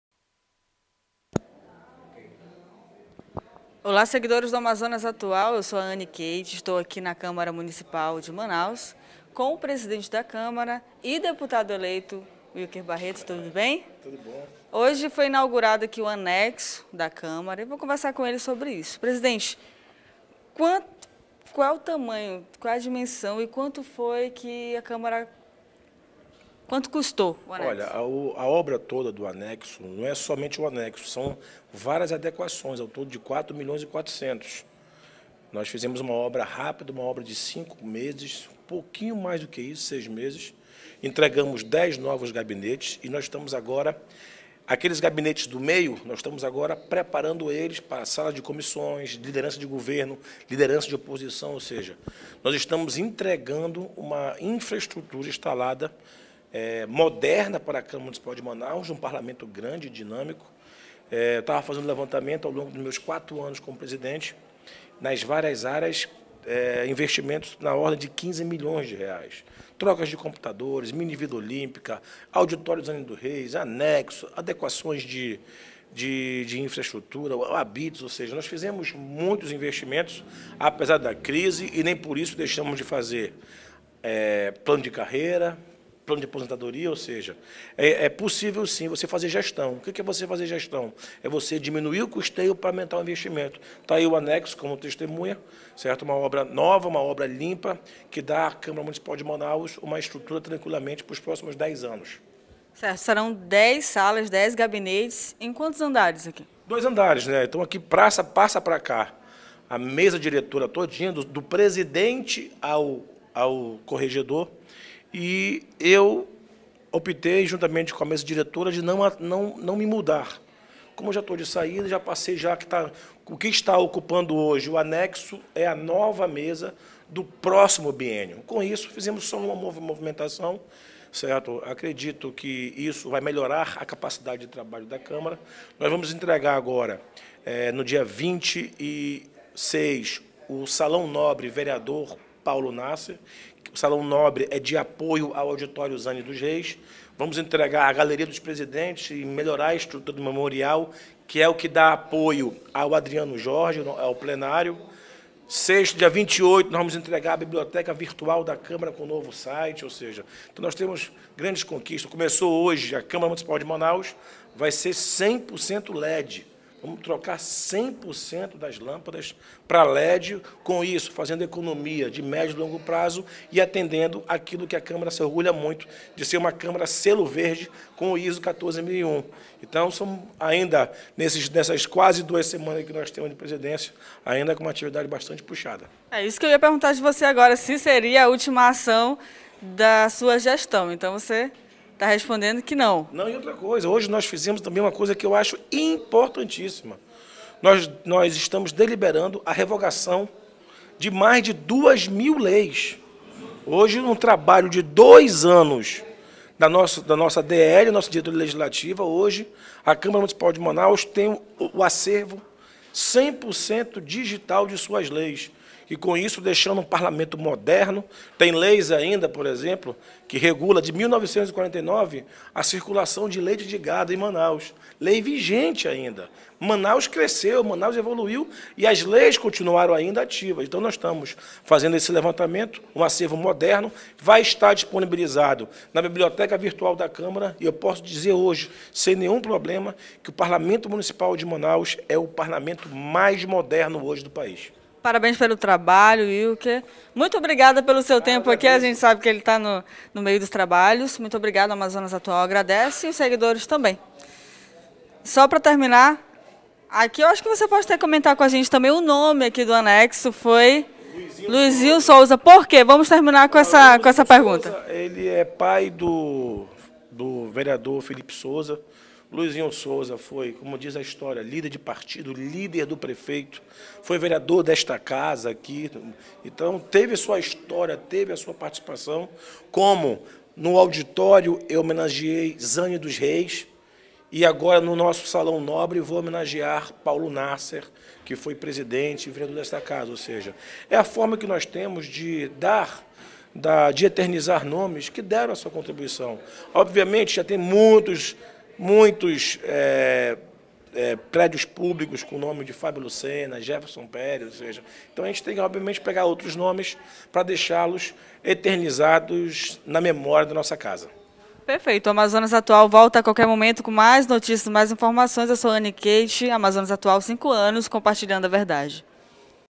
Câmara Municipal de Manaus amplia estrutura
CÂMARA MUNICIPAL DE MANAUS AMPLIA ESTRUTURA | Prédio terá novos gabinetes e departamentos , explica o presidente da Casa Wilker Barreto.